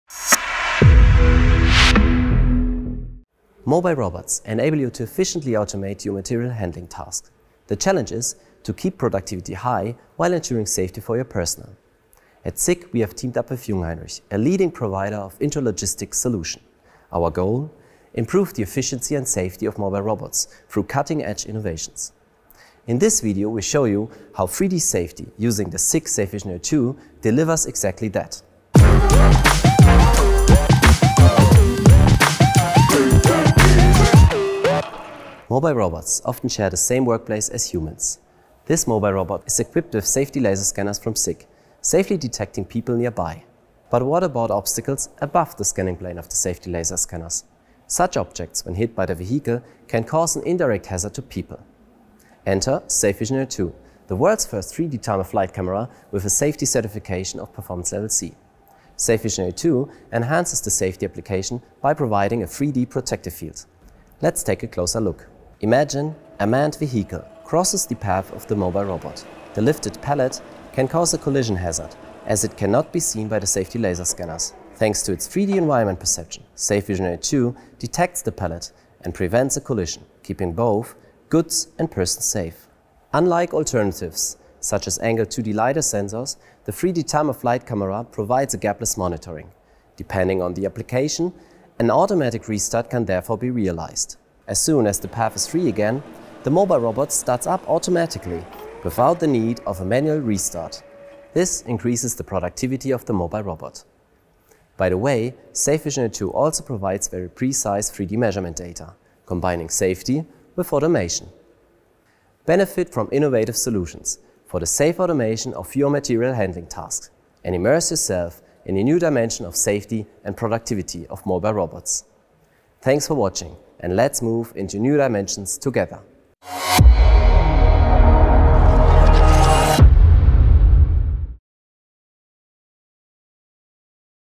safeVisionary2 – Safe 3D camera tackles common AGV problem and helps avoid collisions
How do you avoid collisions between mobile robots and other mobile robots or manned vehicles, e.g., a forklift truck? See in this video how safeVisionary2 adds a secondary level of safety to that provided by safety laser scanners and how it helps avoid damages to personnel and material.
While safety laser scanners are a perfect solution for hazardous area protection, common issue is that they cannot detect obstacles above a certain height, i.e., above the scanners’ scanning plane.
safeVisionary2, the world’s first 3D time-of-flight camera with Performance Level c, tackles this issue by providing a three-dimensional protective field.
In our example, safeVisionary2 detects a pallet transported by a manned forklift truck and prevents a collision by safely bringing the mobile robot to a halt. Due to a gapless monitoring, the application allows an automatic restart, thereby not only increasing safety, but also productivity.
As an added value, precise 3D measurement data delivered by safeVisionary2 help you with your automation tasks.
A big thank you goes to our colleagues from Jungheinrich, with whom we teamed up for this impressive demonstration.
Let’s move into new dimensions together with 3D safety and the safeVisionary2 from SICK.